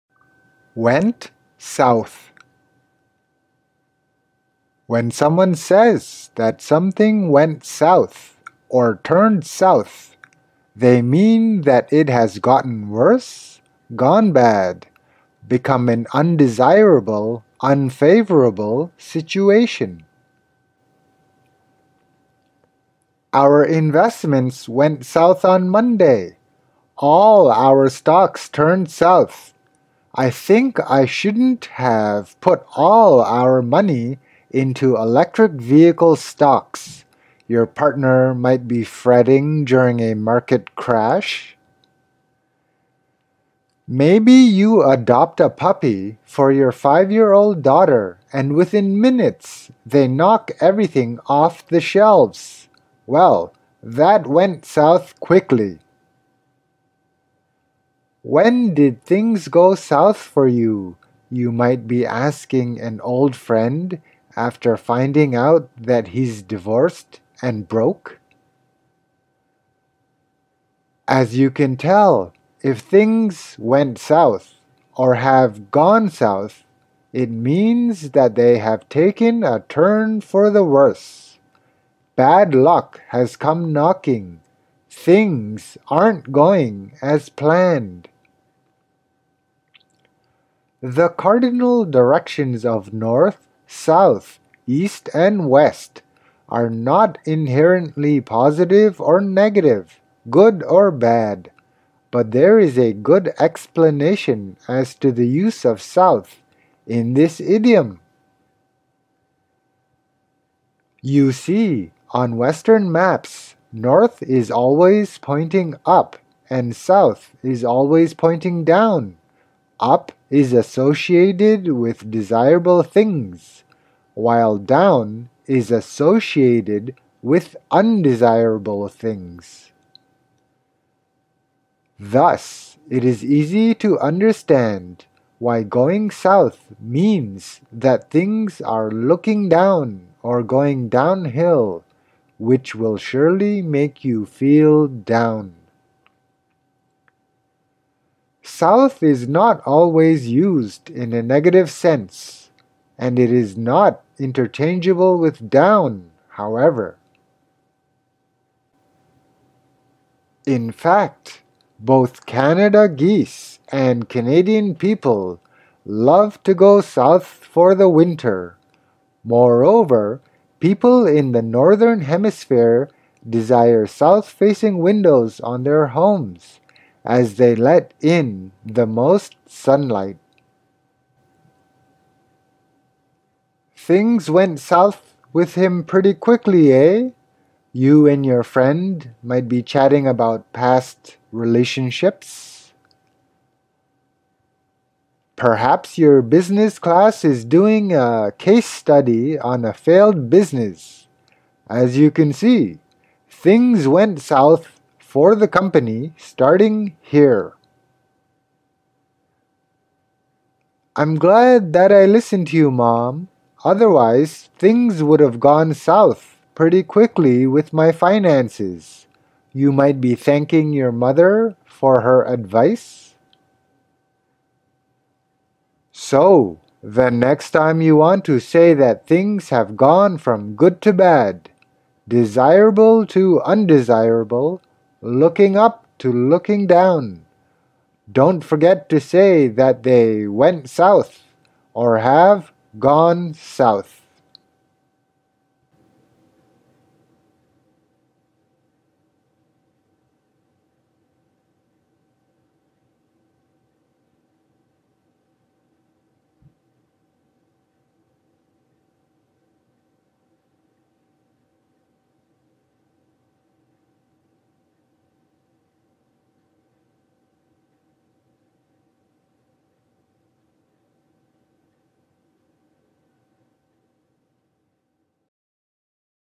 "Went South" Idiom Meaning, Origin & History | Superduper English Idioms
0:00 - How to Say, “Went south…”
0:22 - Examples of “Went south…”
1:10 - Meaning of “Went south…”
1:49 - Origin of “Went south…”
3:02 - More Examples of “Went south…”
3:45 - Conclusion

“Went south…”

When someone says that something “went south,” or “turned south,” they mean that it has gotten worse, gone bad, become an undesirable, unfavourable situation.

“Our investments went south on Monday! All our stocks turned south… I think I shouldn’t have put all our money into electric vehicle stocks.” your partner might be fretting during a market crash.

Maybe you adopt a puppy for your five-year-old daughter and within minutes they knock everything off the shelves, “Well, that went south quickly!”

“When did things go south for you?” you might be asking an old friend after finding out that he’s divorced and broke.

As you can tell, if things “went south” or have “gone south”, it means that they have “taken a turn for the worse”, bad luck has come knocking, things aren’t going as planned.

The cardinal directions of north, south, east and west are not inherently positive or negative, good or bad, but there is a good explanation as to the use of “south” in this idiom!

You see, on Western maps, north is always pointing up and south is always pointing down; “up” is associated with desirable things, while “down” is associated with undesirable things.

Thus, it is easy to understand why going south means that things are looking down or going downhill, which will surely make you feel down.

South is not always used in a negative sense (and it is not interchangeable with “down”), however…

… in fact, both Canada geese and Canadian people love to go south for the winter! Moreover, people in the Northern Hemisphere desire south-facing windows on their homes, as they let in the most sunlight.

“Things went south with him pretty quickly, eh?” you and your friend might be chatting about past relationships.

Perhaps your business class is doing a case study on a failed business, “As you can see, things went south for the company starting here…”

“I’m glad that I listened to you, mom, otherwise things would’ve gone south pretty quickly with my finances,” you might be thanking your mother for her advice.

So, the next time you want to say that things have gone from good to bad, desirable to undesirable, looking up to looking down, don’t forget to say that they “went south” or have “gone south”!